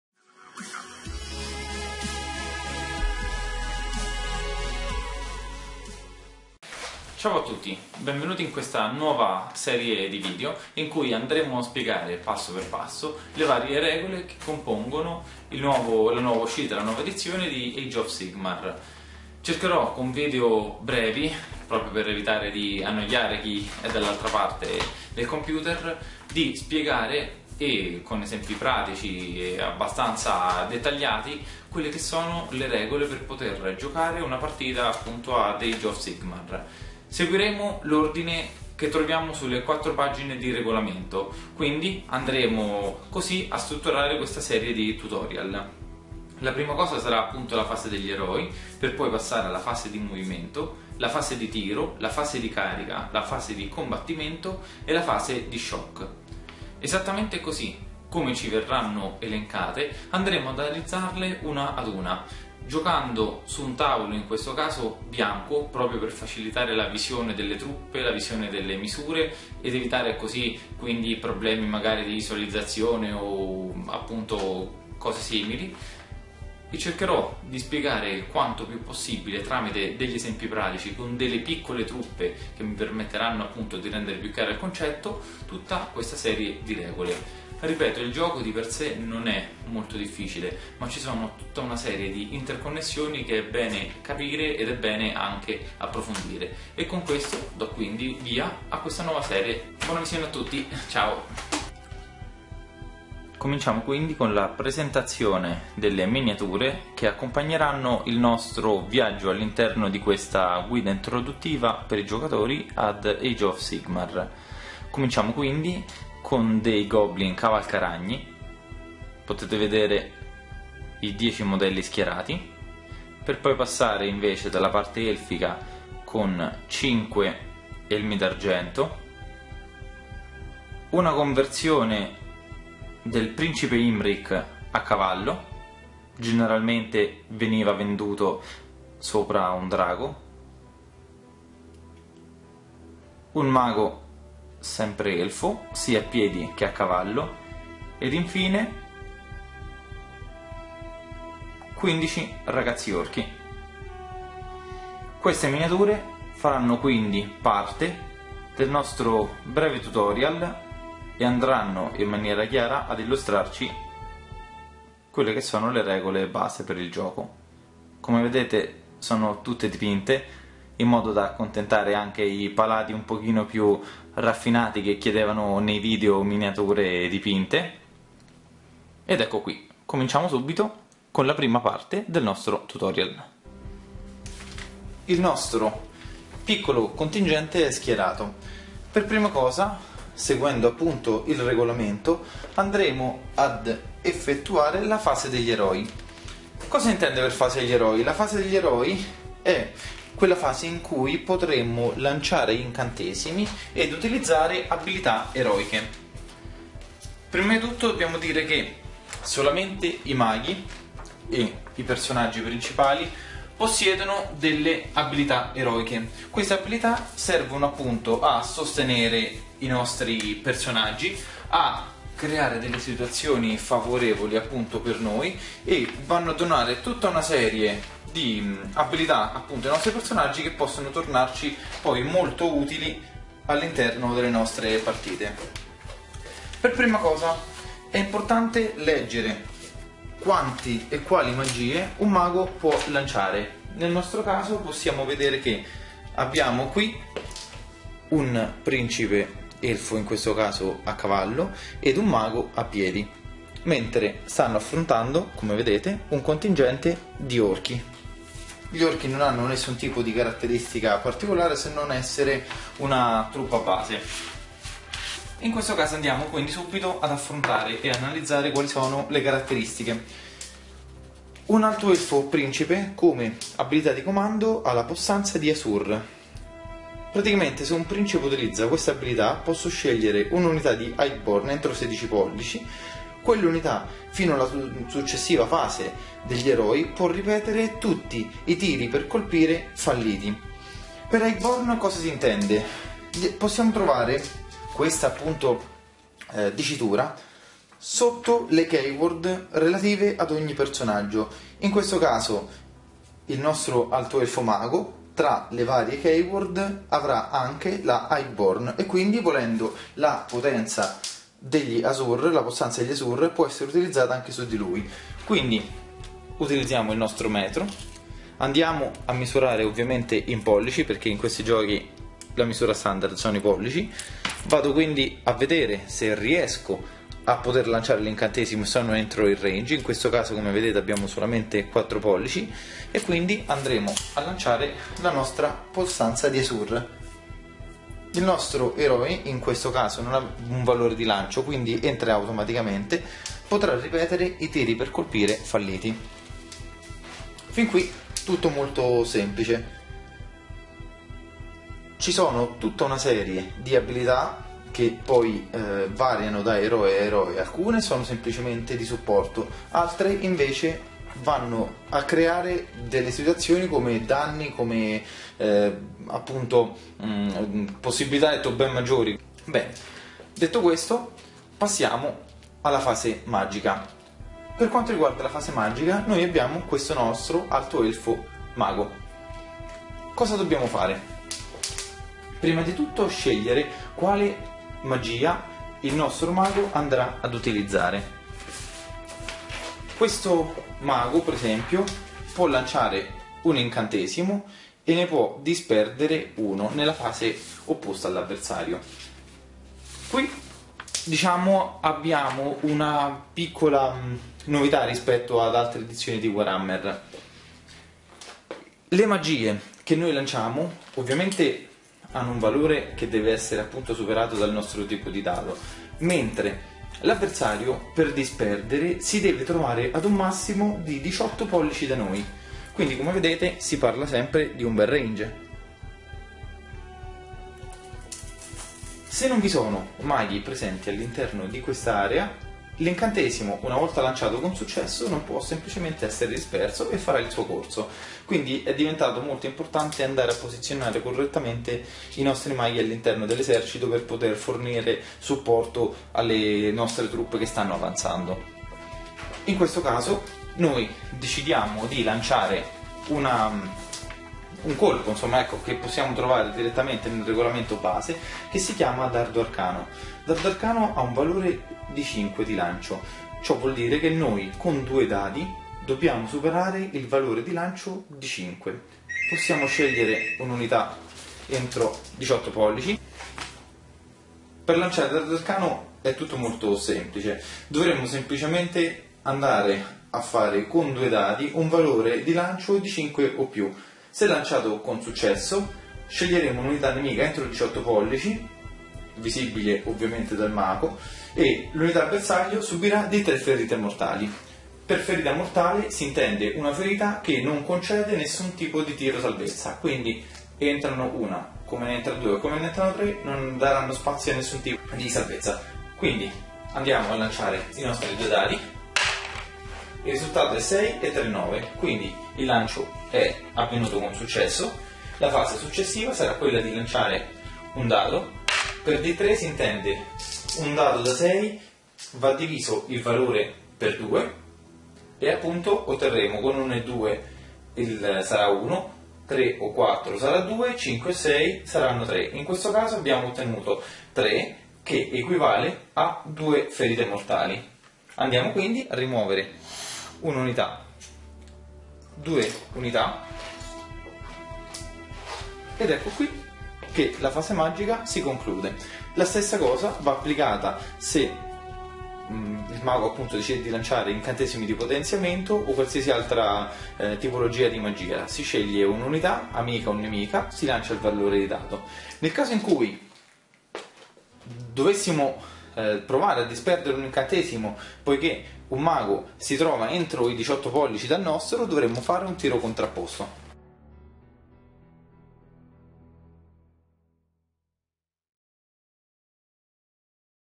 Age of Sigmar Rules - Abilità e Magia - Parte 1
Parte 1

In questo video andremo a spiegare come preparare il campo di battaglia in Age of Sigmar andando ad approfondire le varie dinamiche di Gioco.

Info Fotografiche

Canon 7D + Tamron SP 17-50mm F/2.8 XR Di II VC LD

Canon 450D + 18-55 basic